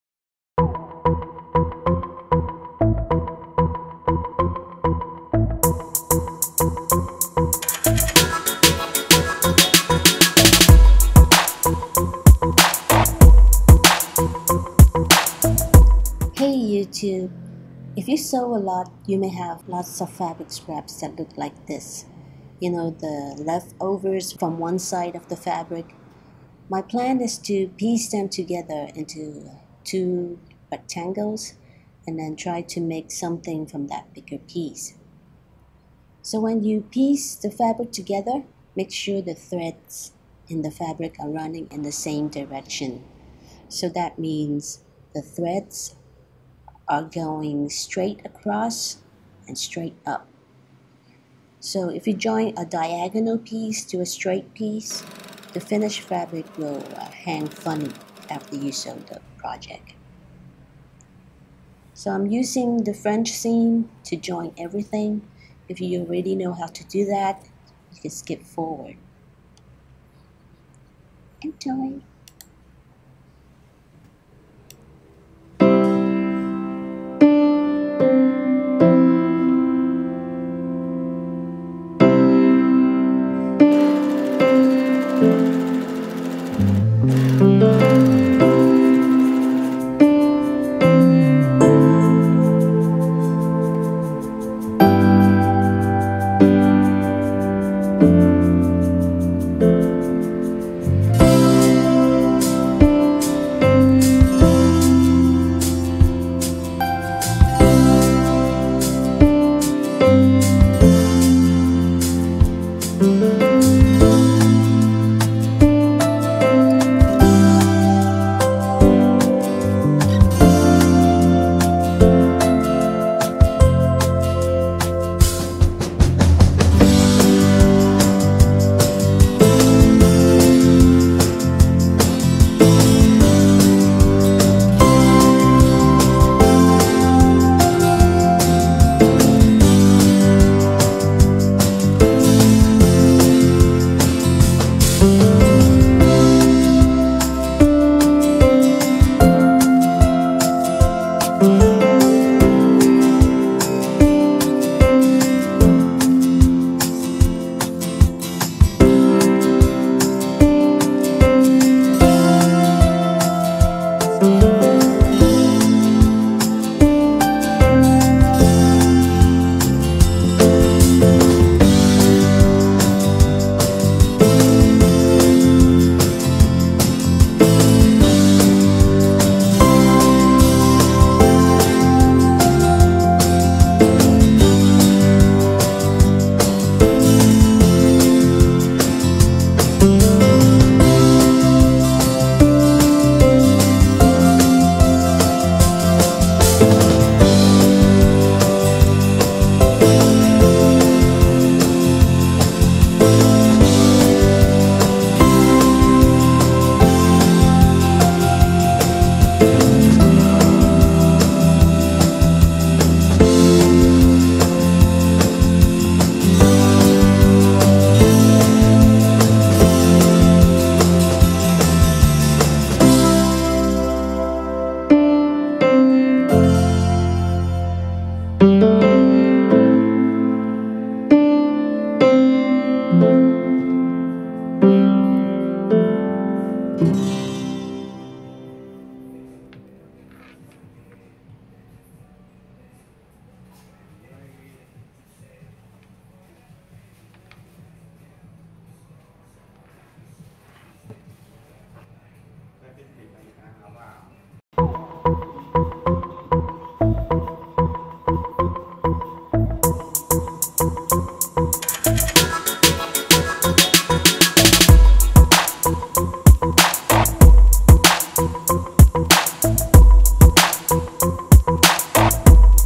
Fabric Scrap Sewing Project: Leather Strap Tube Apron Tutorial
Sew a tube style apron with fabric scraps. The leather straps can be taken off and the apron, washed. It's made from 2 rectangles !